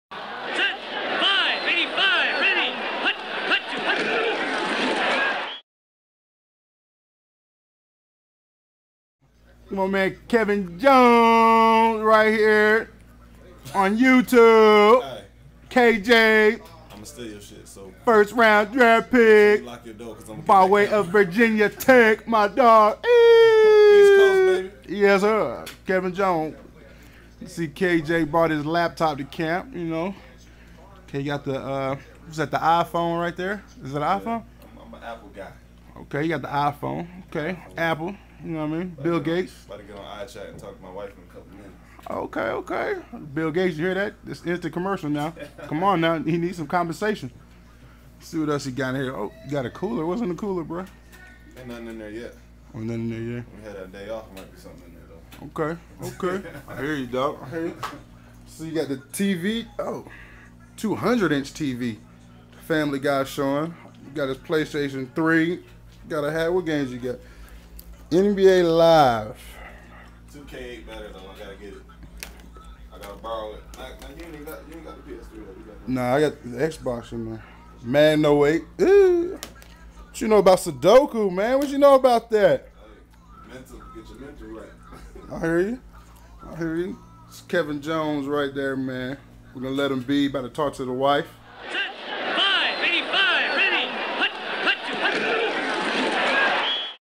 Athlete TV-Chicago Bears Fullback Jason McKie: Sudoku with Running Back Kevin Jones
What you know 'bout Sudoku? Bears running back Kevin Jones might know something. Watch as fellow Bear Jason McKie peeks around his room.